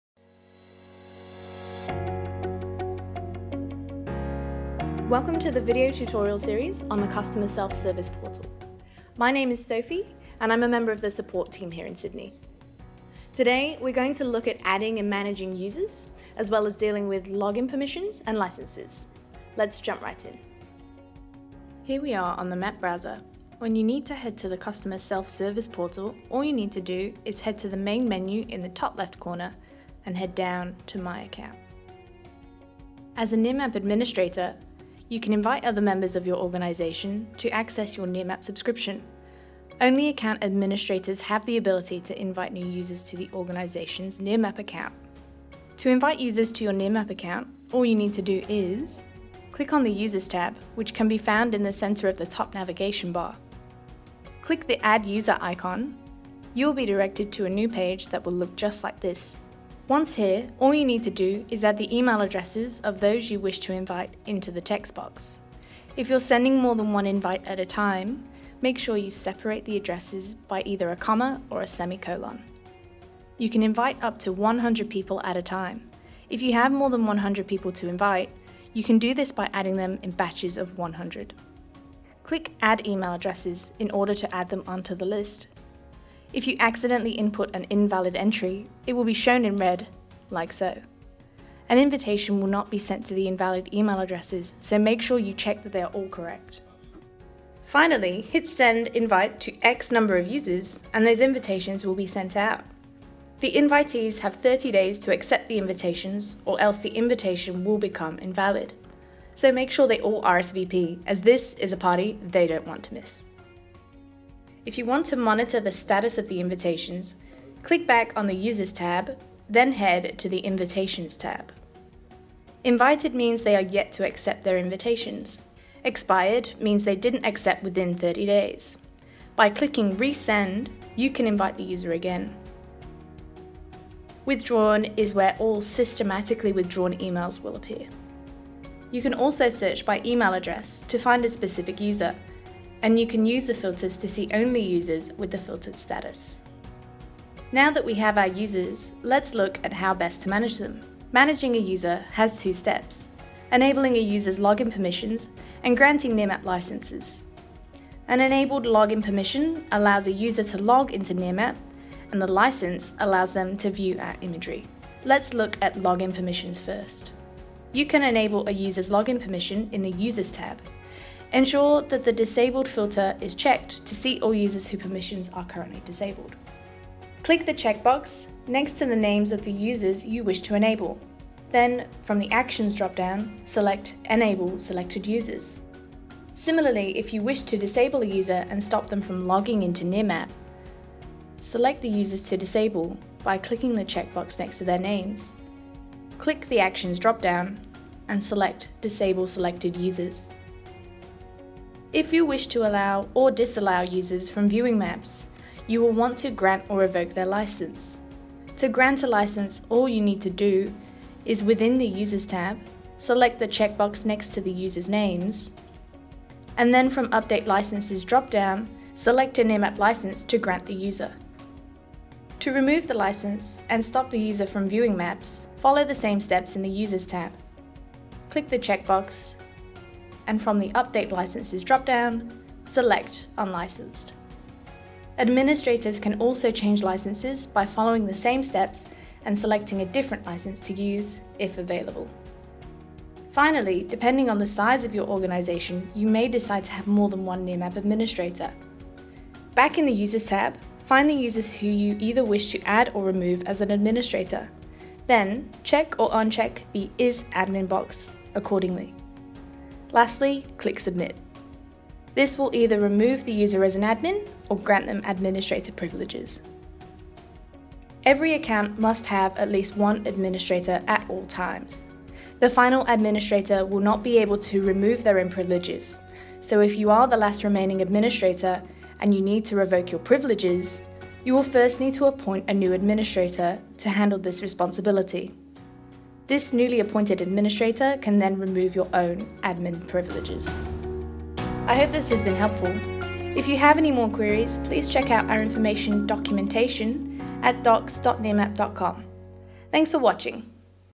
Nearmap Service Portal - Part 1 - How to Add and Manage Users
In this short tutorial, you will learn how to add and manage users, login permissions and licences all on our customer Service Portal.